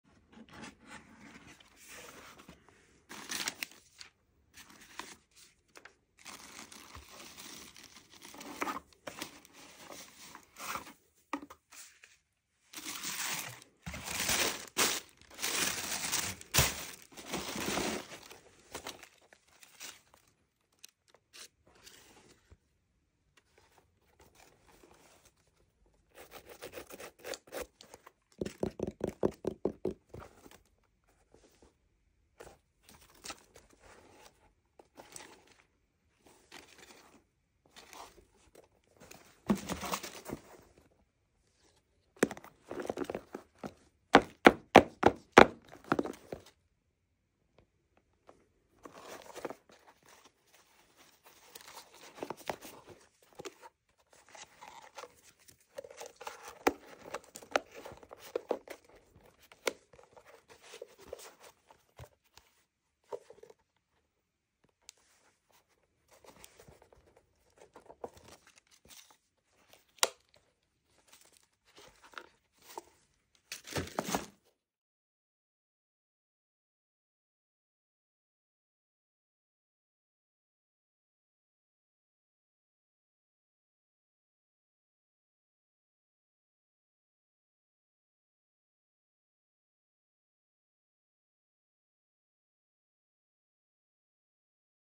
Unboxing the new 2024 Adidas Predator Elite Fold Over Tongue Firm Ground football boots
These boots are simply unbelievable 😍